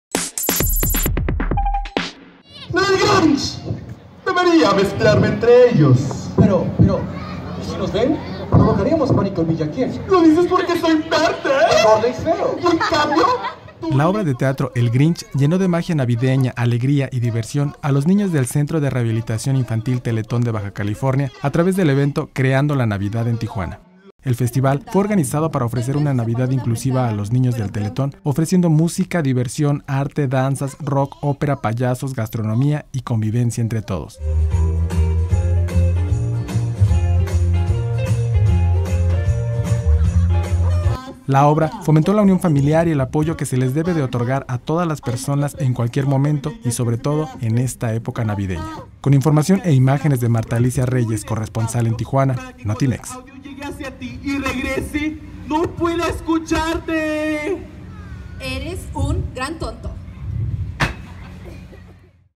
Obra "El Grinch" llena de magia navideña a niños del Teletón en Tijuana
La obra, fomentó la unión familiar y el apoyo que se les debe de otorgar a todas las personas en cualquier momento y sobre todo en esta época navideña